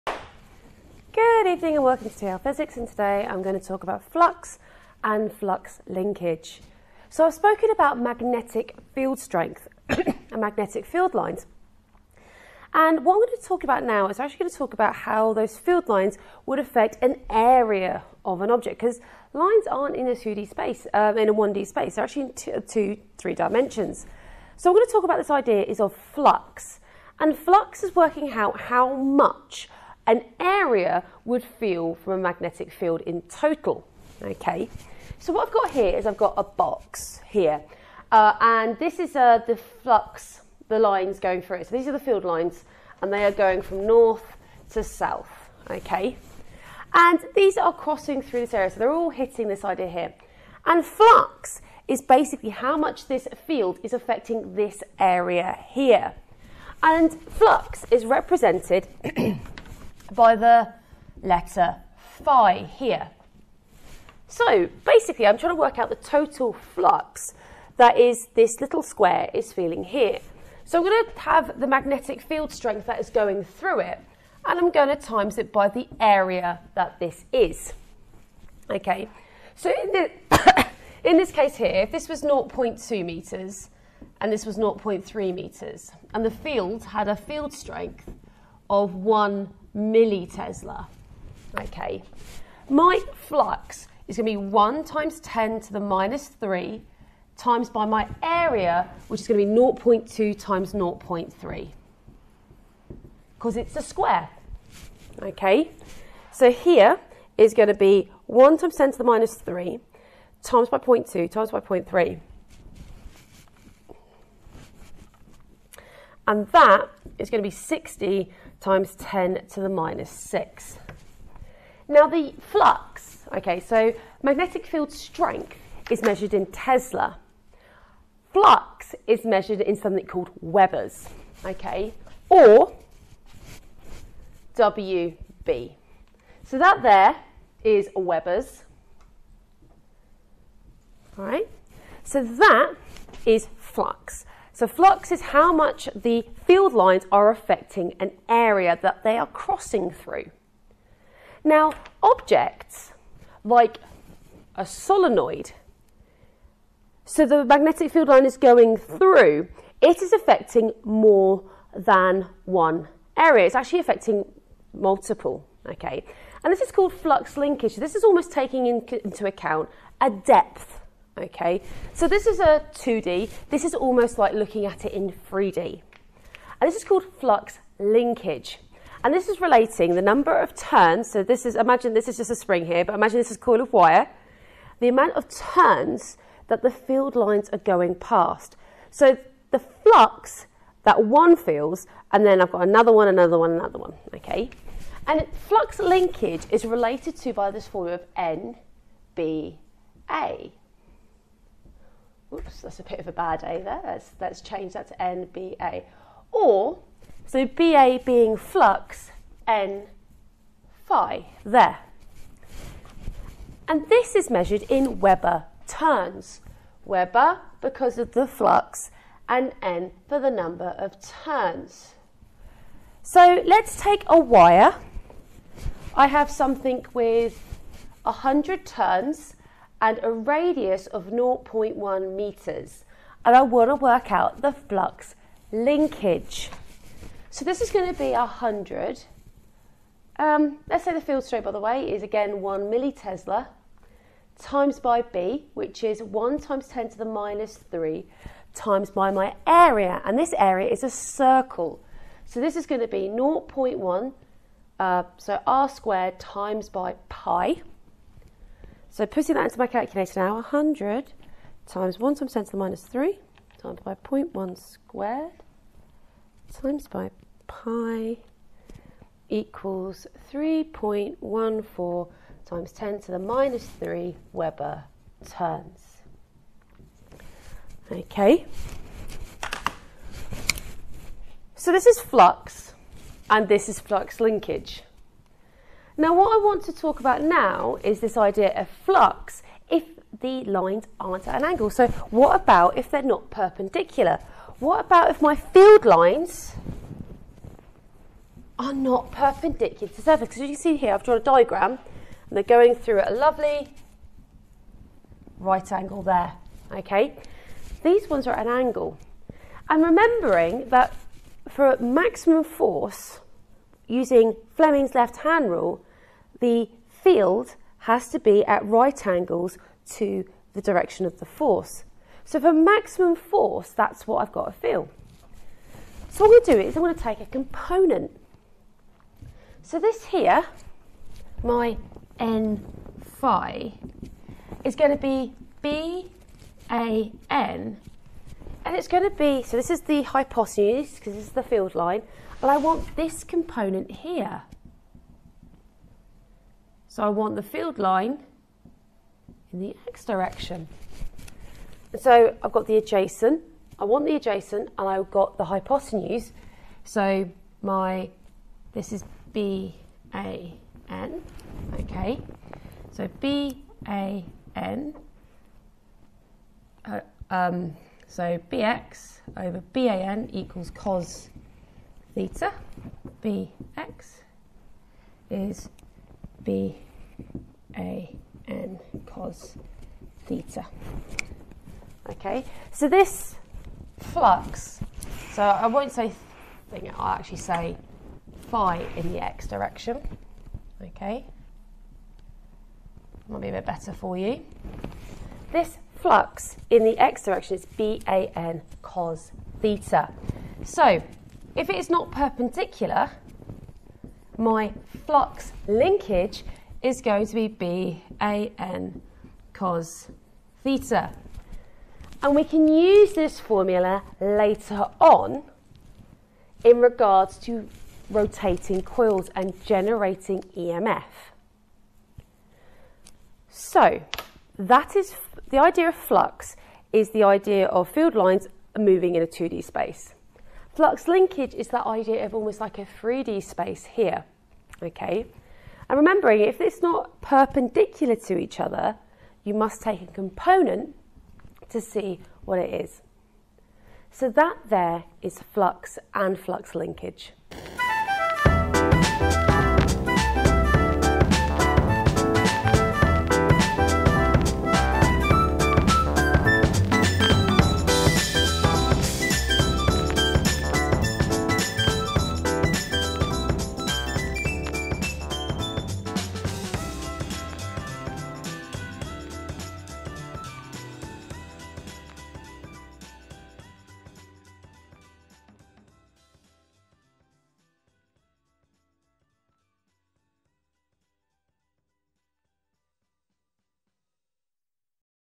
Flux and flux linkage AQA Alevel Physics
How magnetic fields work over an area and a volume